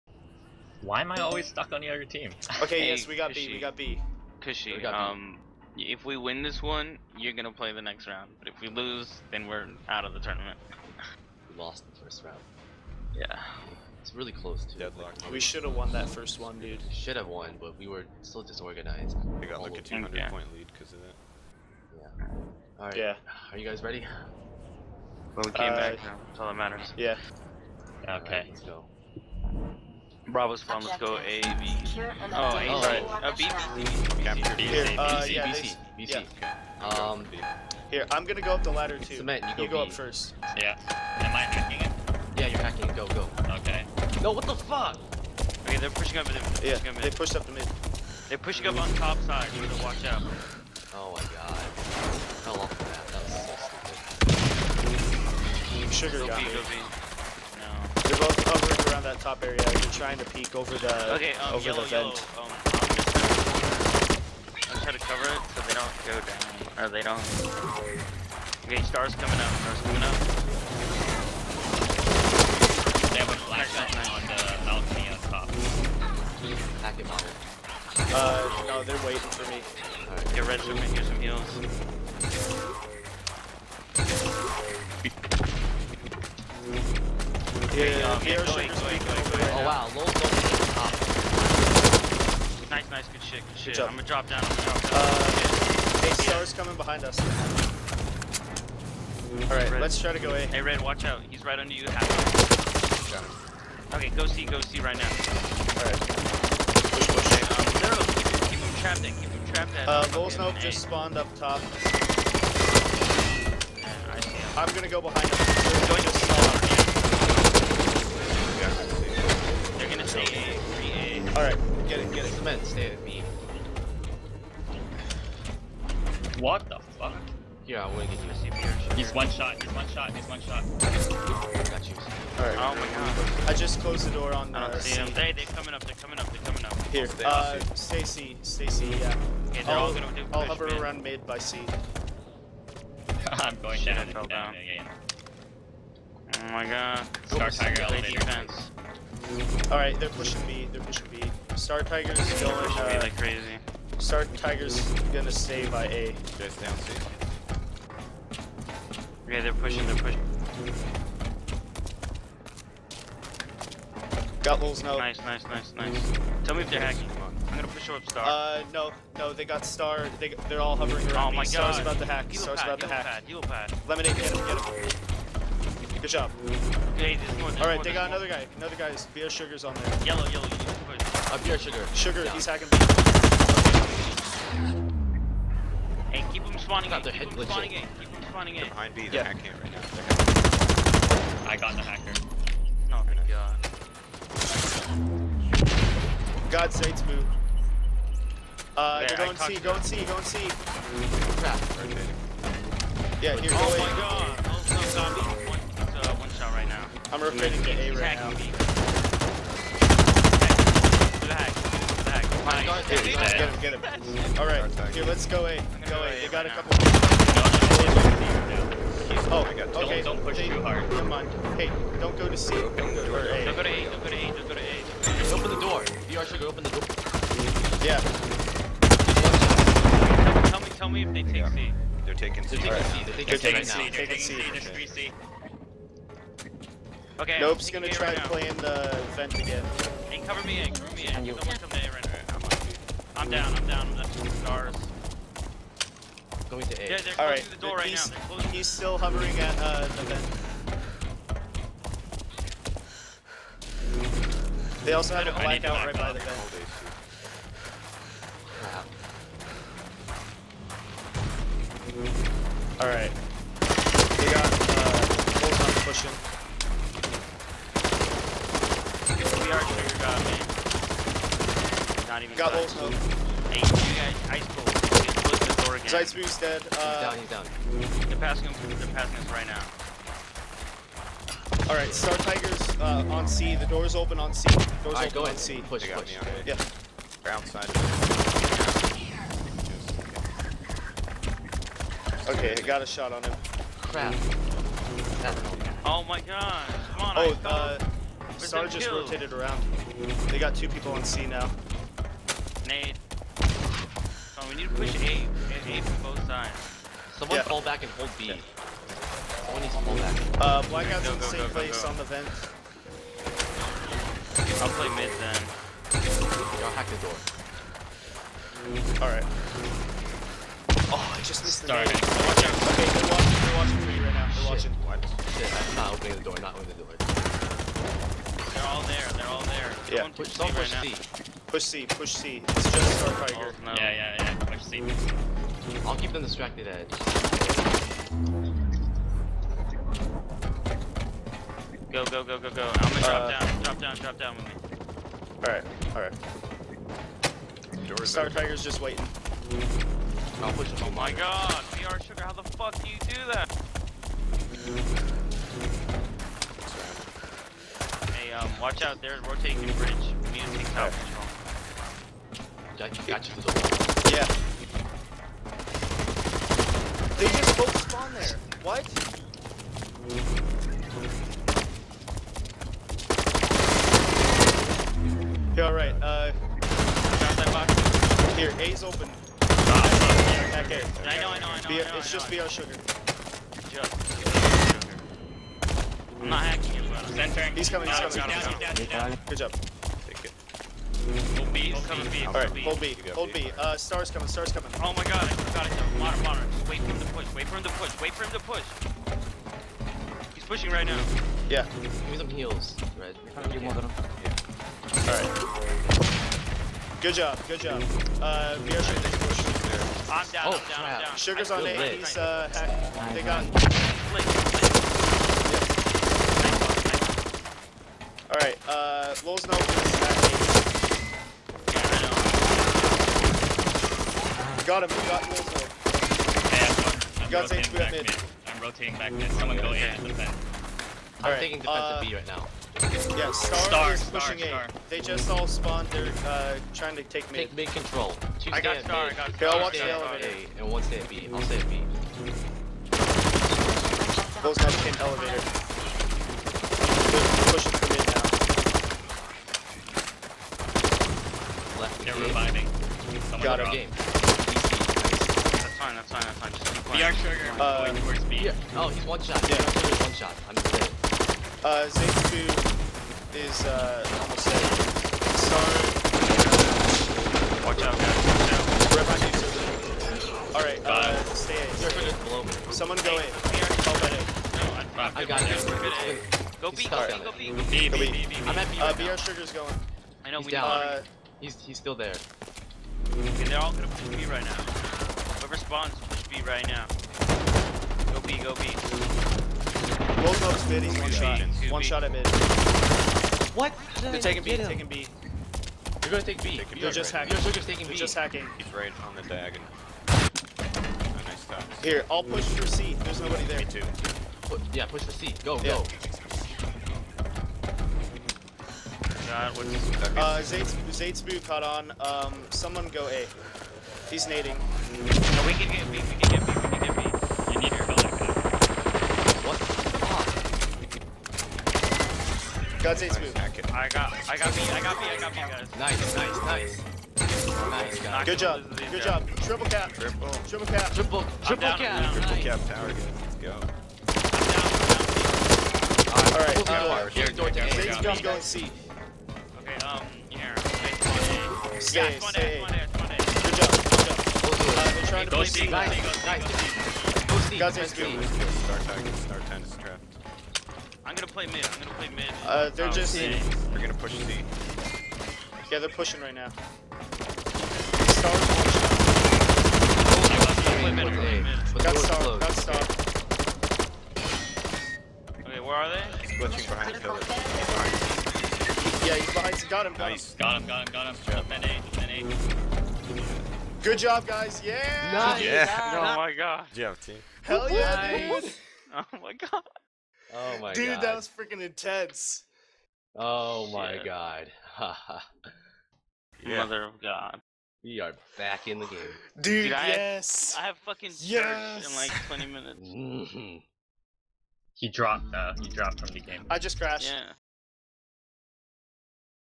BCL Winter Cup - CLR vs StaR Vertigo
Second game of the series we played against StaR.
Good game to both teams!